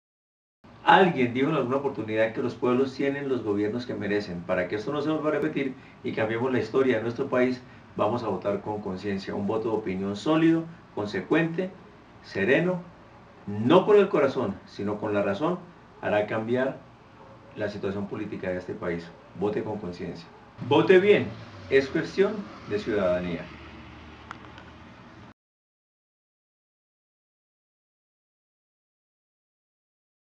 Vote bien, vote con la razón ...
Video de apoyo a la campaña vote bien, es cuestión de ciudadanía.